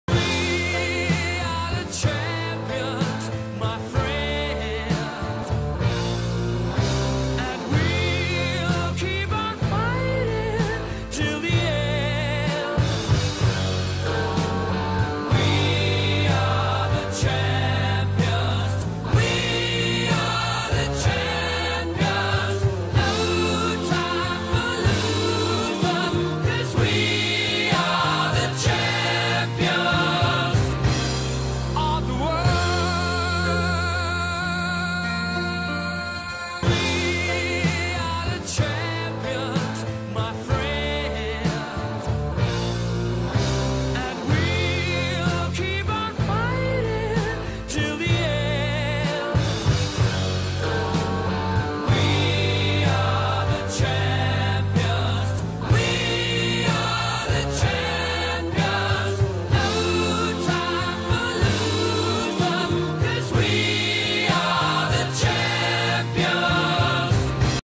Pep Rally for Equestrian Team and Tennis
Congradulations to the Equestrian Team. Along with Mackenzie and Erin for placing 4th at state for Tennis!